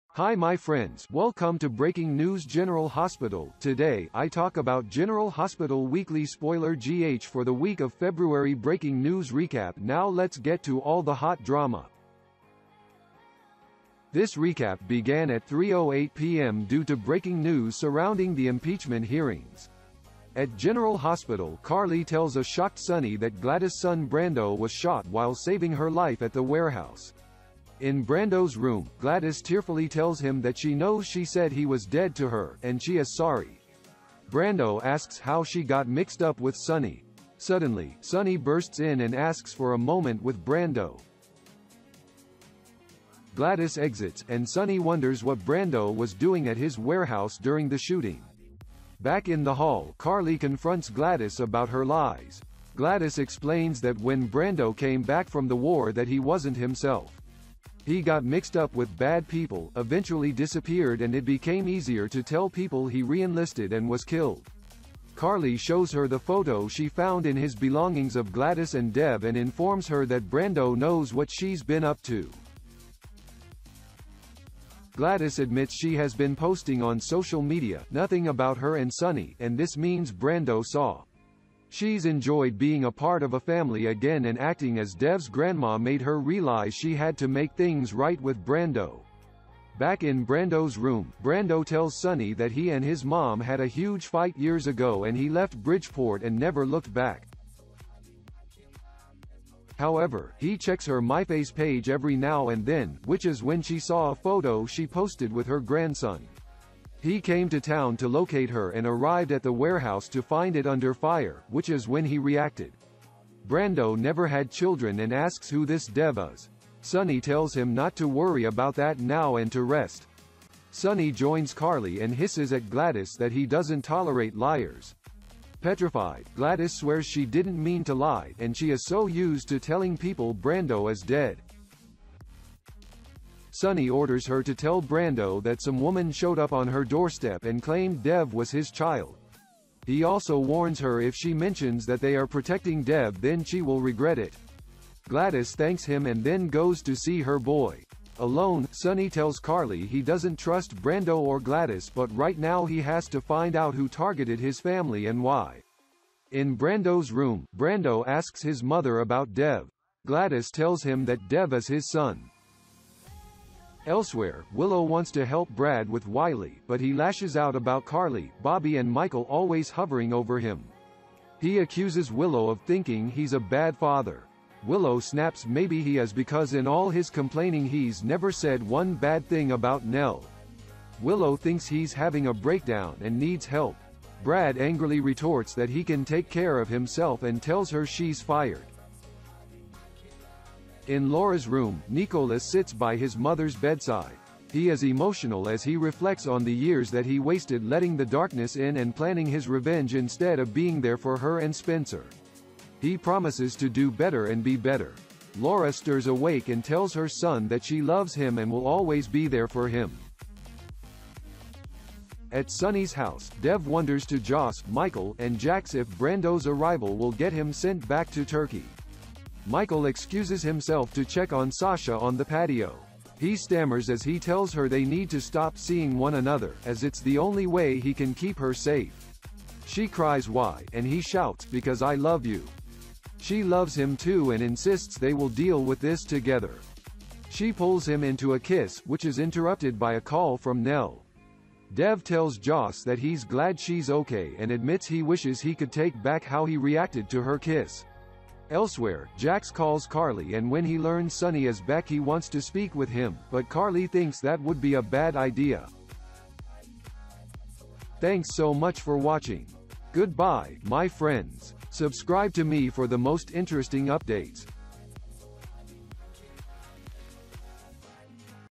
General Hospital REVEAL: Breaking News Recap - February 5/2020
Carly tells a shocked Sonny that Gladys’ son Brando was shot while saving her life at the warehouse. In Brando’s room, Gladys tearfully tells him that she knows she said he was dead to her, and she is sorry. Brando asks how she got mixed up with Sonny. Suddenly, Sonny bursts in and asks for a moment with Brando.